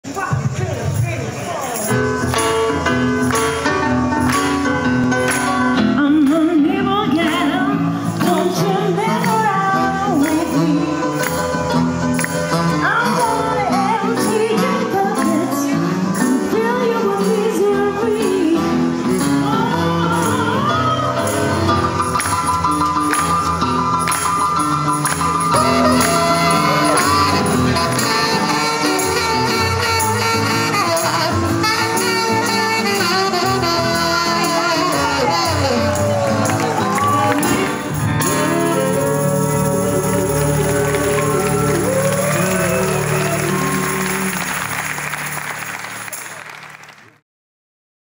Noches de Blues en el Poble Espanyol
Las noches de los miércoles se llenan de los inspiradores ritmos del mejor blues de nuestro país en el Poble Espanyol.

Cuando el sol se pone, las bandas más destacadas del panorama nacional empiezan a tocar los primeros acordes para ofrecer desde el blues más melódico hasta los estilos más bailables. Todo en un entorno agradable y tranquilo… ¿Se te ocurre un plan mejor para los atardeceres de los miércoles de julio y agosto?

Este año son diez actuaciones de pequeño formato las que integran la programación de las Noches de Blues, una cuidada selección de artistas que a través de su pasión harán las delicias de los amantes de la buena música de todas las edades.